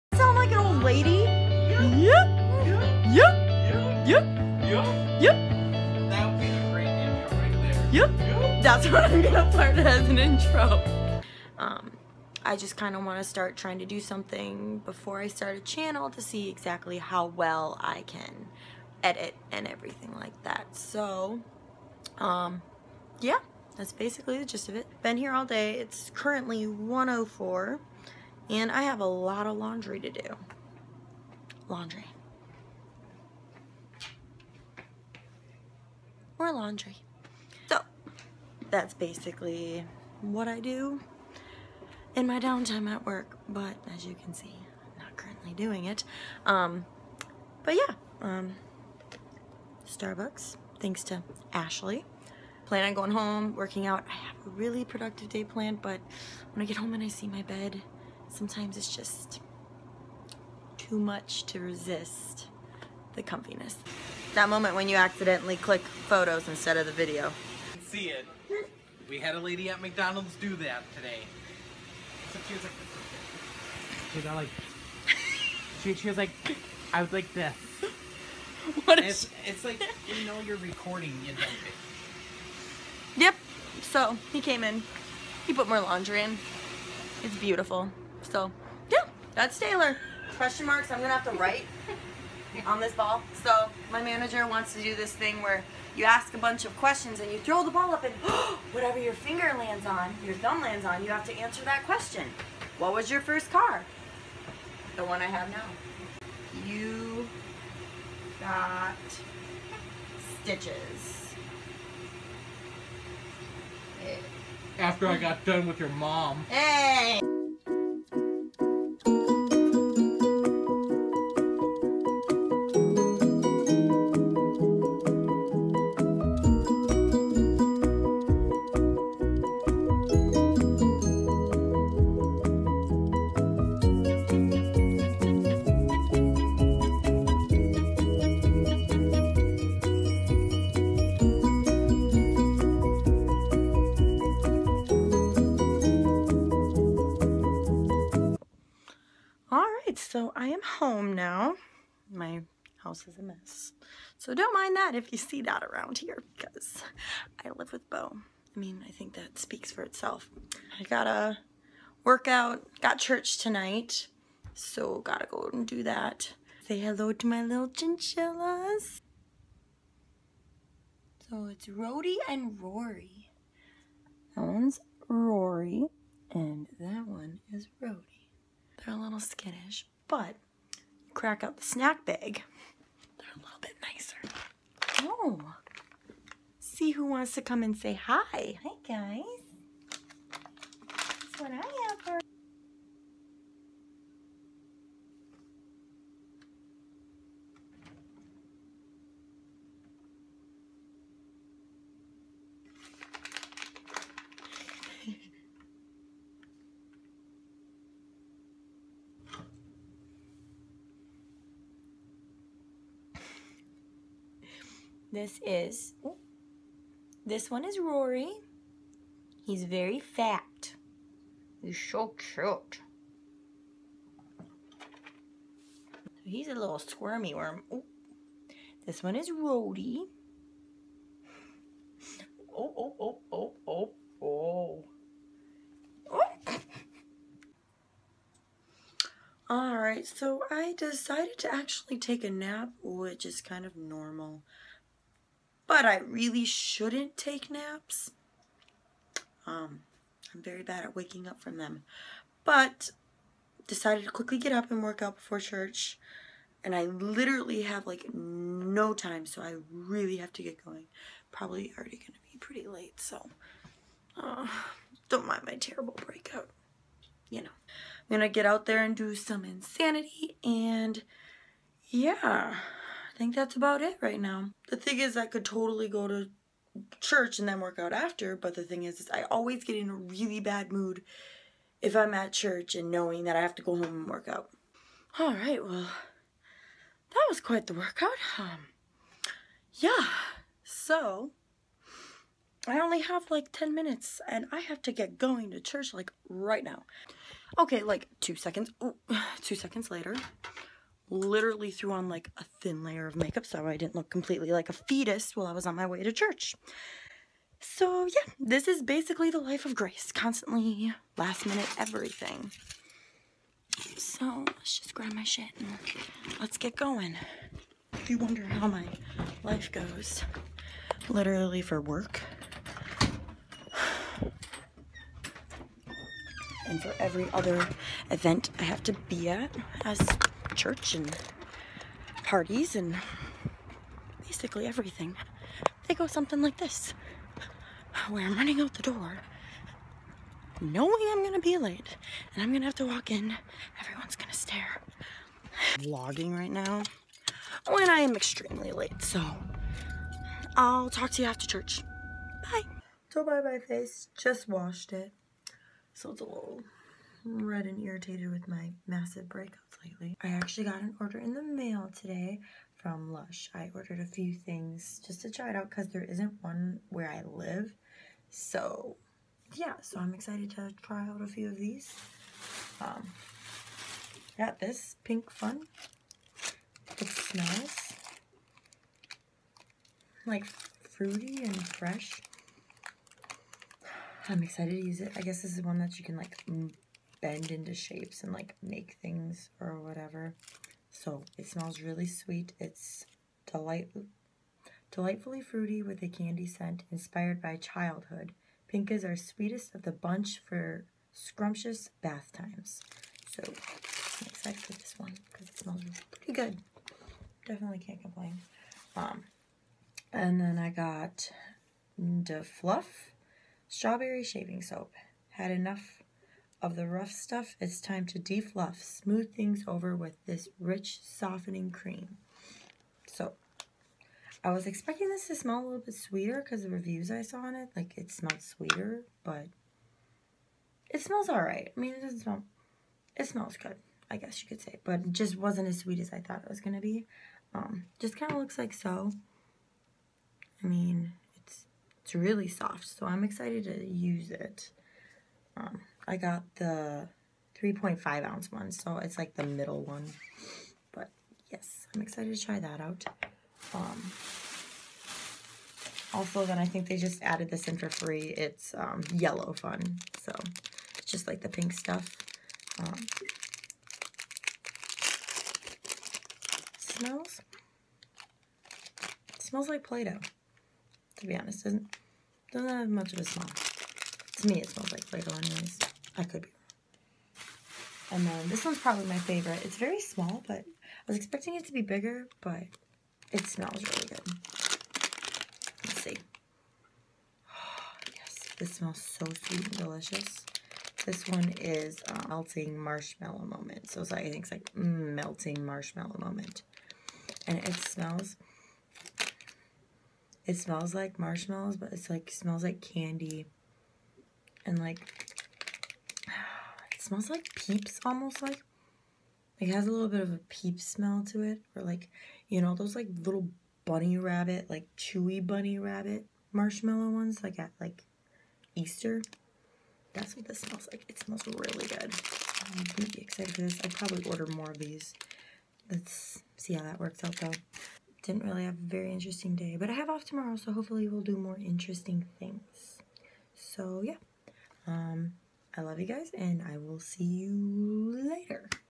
WORK, MEET MY CHINCHILLAS AND CHURCH!
Meet my chinchillas! GUYS OMG This is my first vlog ever! I'm really excited to start my youtube journey. Today was a really chill day, just work and a few things to do.